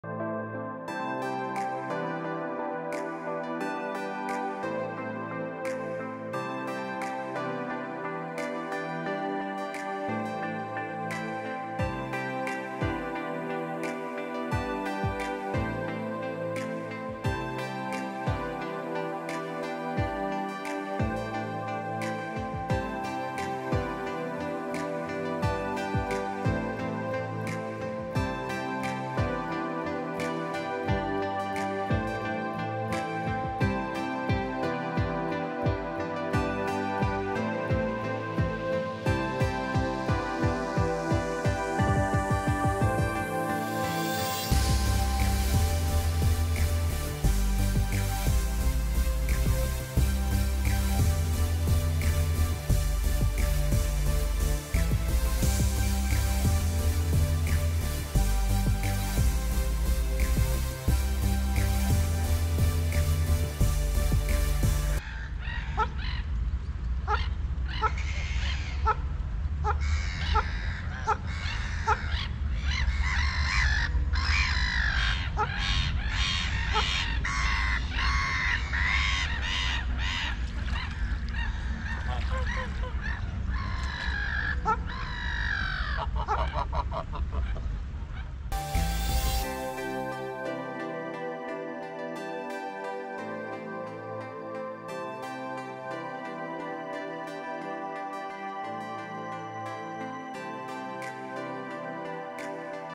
Birds Flying High Its Amazing..
This bird video is compilations of beautiful Birds habitats and geography. They are beautiful creature on our earth . Nature is truly full of surprise !